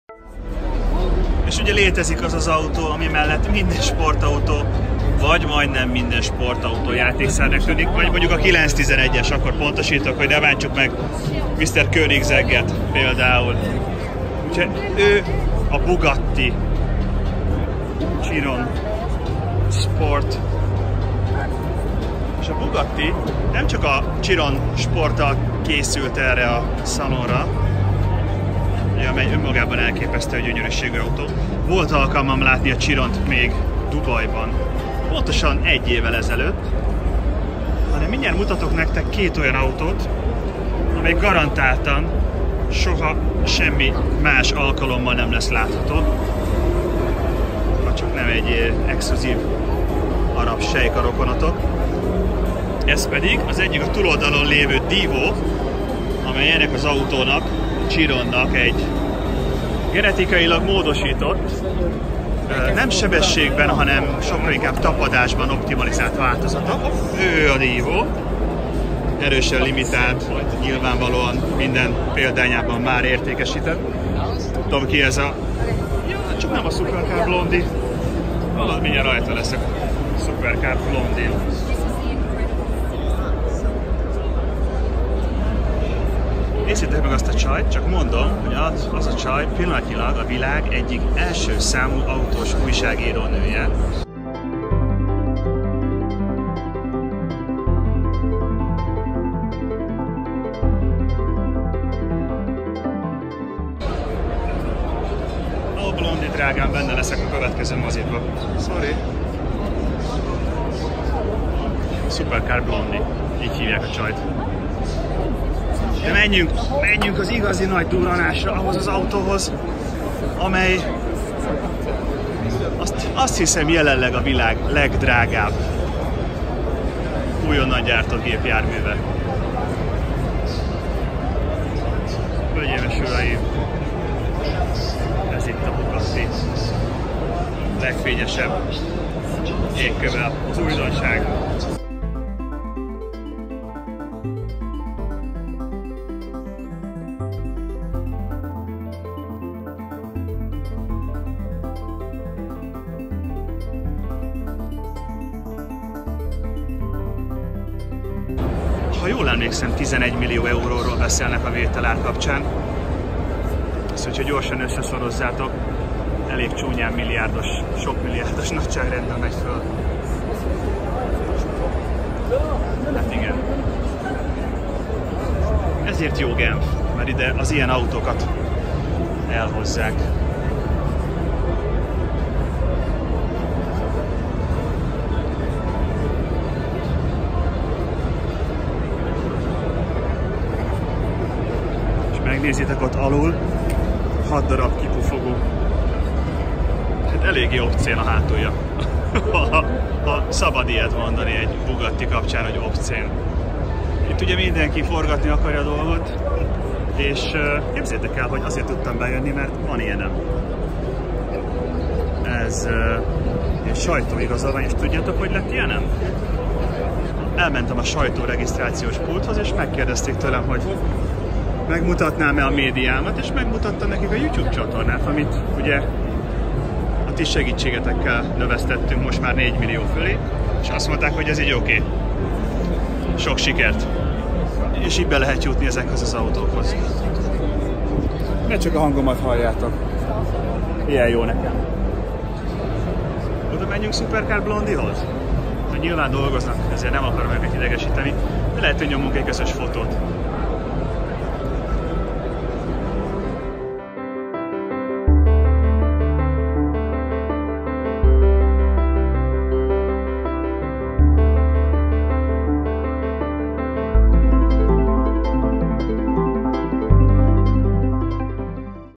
Bugatti Chiron Sport, Divo és a La Voiture Noire, Ára: 5 millárd forint, meg az apró - AutóSámán
Igen, van, hogy benézem, elrontom. Nem Csiron, hanem Siro(n). Hamut szórok a fejemre. Kedves Bugatti, Chiron Család az Ég Áldja Önöket.

Álljon itt pár gondolat Chiron-ról.

Louis Alexandre Chiron (3 August 1899 – 22 June 1979) was a Monégasque racing driver who competed in rallies, sports car races, and Grands Prix. He is the oldest driver ever to have raced in Formula One, having taken 6th place in the 1955 Monaco Grand Prix when he was 55.

Genf legdrágább autóit a Bugatti standon láthatjuk. Kihasználva a lehetőséget bejutottam a kordonon belülre. Supercar Blondie csak a hab volt a Bugatti La Voiture Noir (azaz a Fekete Autó) tortáján. Végül maradtam az autóknál. Bye, Babe!

Nézd meg ezt is: "Szívnek szól. Elcsábít? Peugeot 508 AutósMozi - AutóSámán"